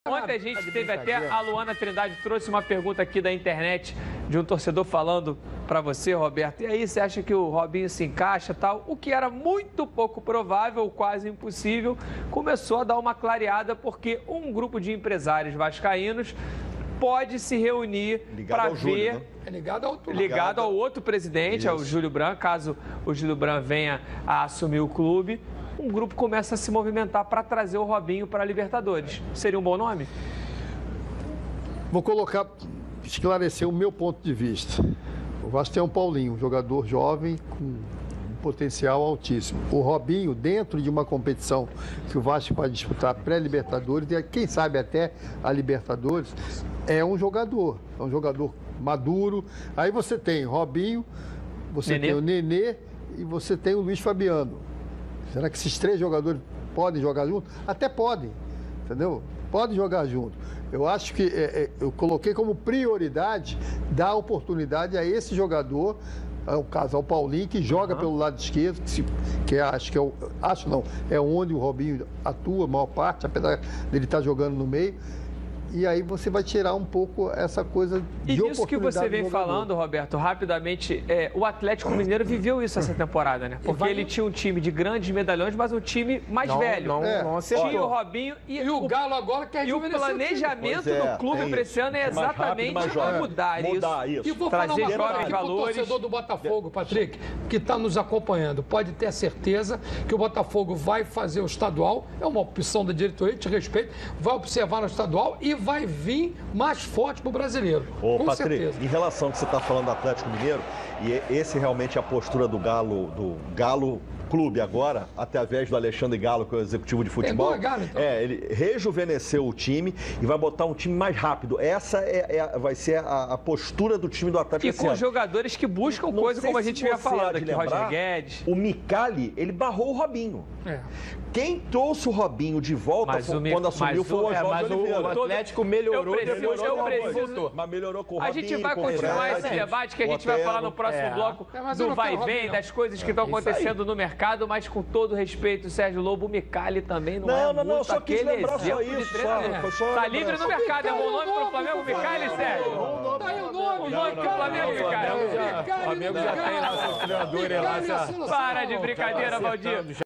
Robinho pode ser o novo reforço do Vasco
Grupo de empresários pode ajudar o presidente eleito, Julio Brant a trazer o jogador para o Vasco.